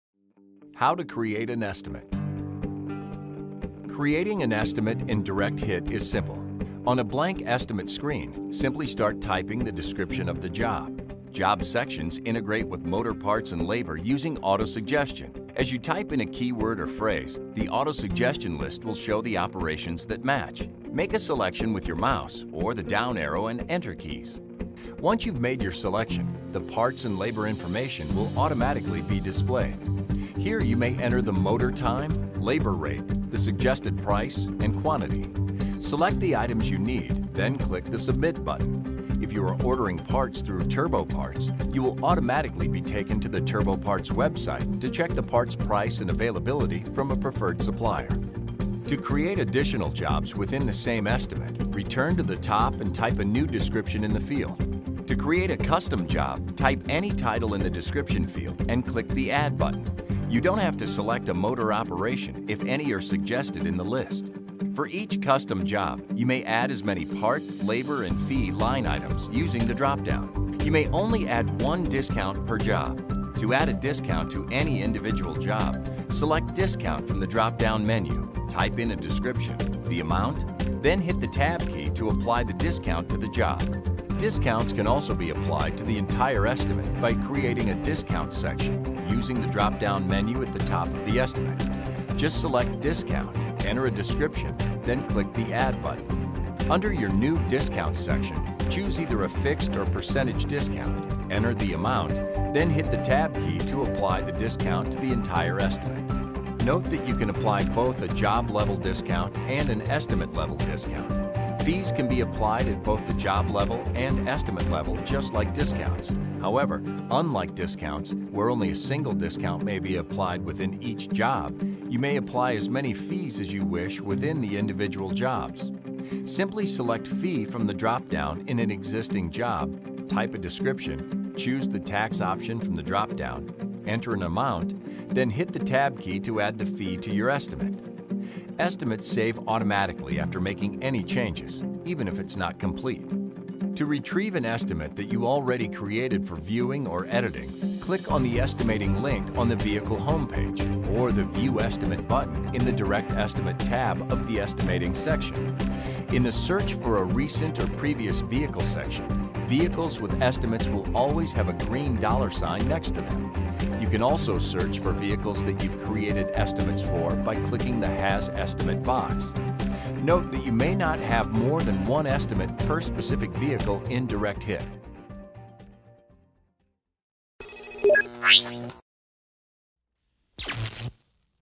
Identifix: How to Create an Estimate in Direct-Hit
Watch as this Identifix video explains exactly how to create a customer estimate within Direct-Hit. You'll learn how to add multiple jobs within a vehicle service estimate, create estimate templates and more.